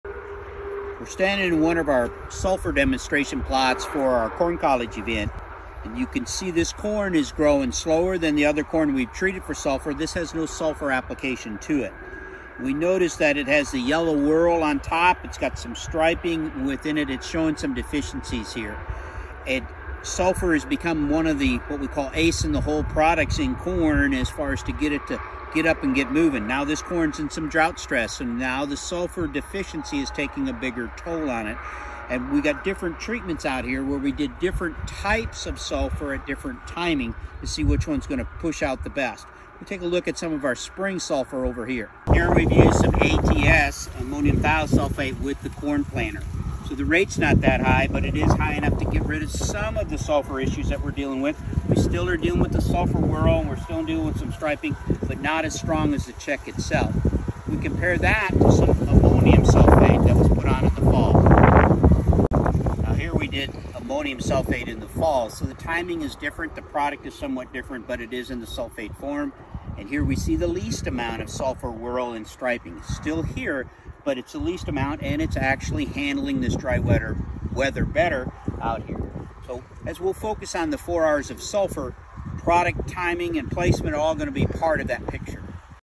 Know How to Best Utilize Your Sulfur Source
Timing, rate, product, and placement of sulfur play a huge part in the outcome. Ken walks through strips of no sulfur, an ATS application, and an AMS application to show how changing timing, product, and rate can change the outcome not only for sulfur uptake but also how plants handle drought stress.